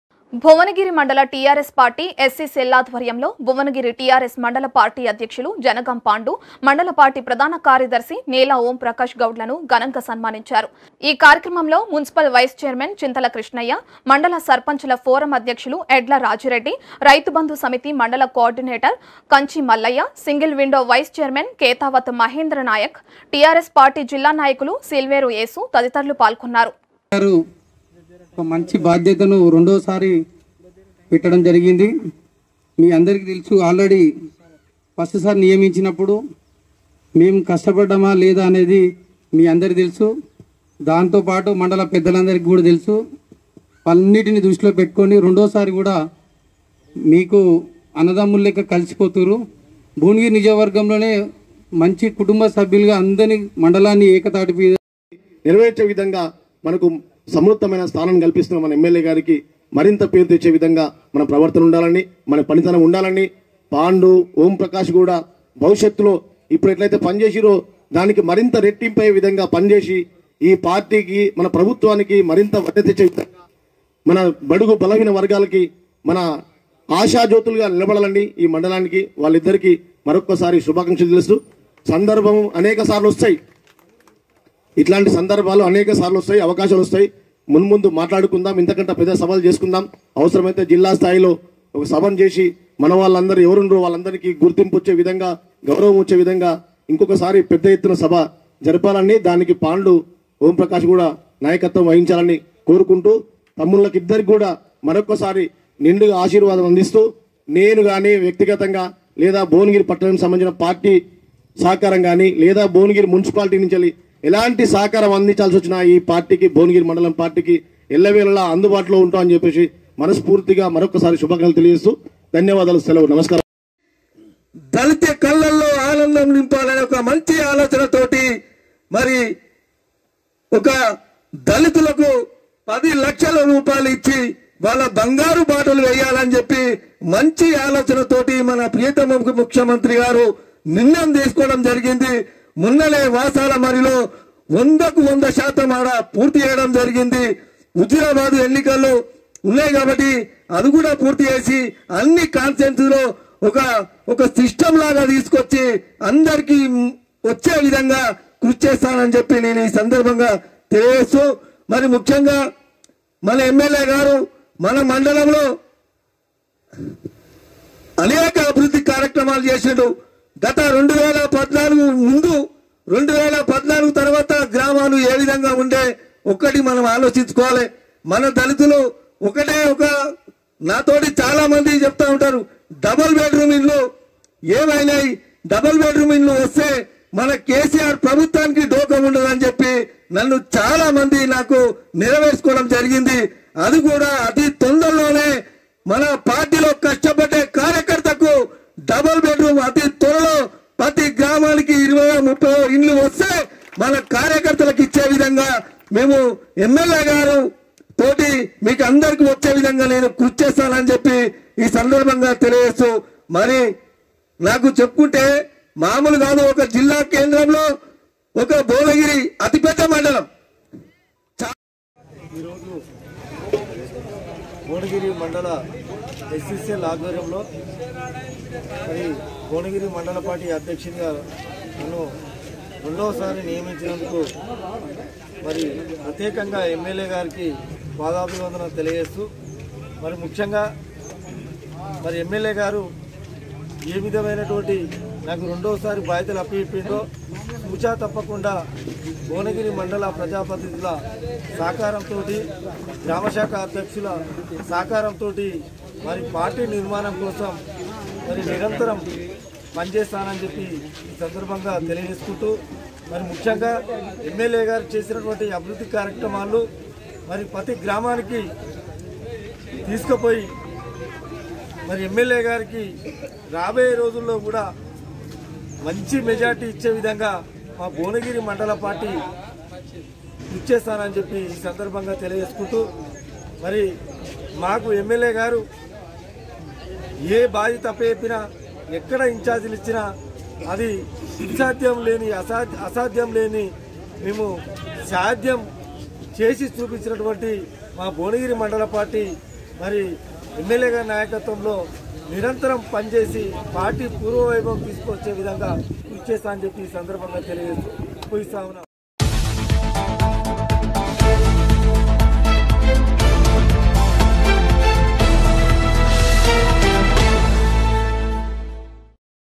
భువనగిరి టిఆర్ఎస్ మండల పార్టీ అధ్యక్షులు జనగాం పాండుకి సన్మానం | b6 news
భువనగిరి మండల టిఆర్ఎస్ పార్టీ ఎస్.సి సెల్ ఆధ్వర్యంలో.. భువనగిరి టిఆర్ఎస్ మండల పార్టీ అధ్యక్షులు జనగాం పాండు, మండల పార్టీ ప్రధాన కార్యదర్శి నీల ఓంప్రకాశ్ గౌడ్‌లను ఘనంగా సన్మానించారు. ఈ కార్యక్రమంలో మున్సిపల్ వైస్ చైర్మన్ చింతల కిష్టయ్య,మండల సర్పంచుల ఫోరం అధ్యక్షులు ఎడ్ల రాజిరెడ్డి, రైతుబంధు సమితి మండల కోఆర్డినేటర్ కంచి మల్లయ్య, సింగిల్ విండో వైస్ చైర్మన్ కేతావత్ మహేంద్ర నాయక్, టిఆర్ఎస్ పార్టీ జిల్లా నాయకులు సిల్వేరు ఏసు, తదితరులు పాల్గొన్నారు.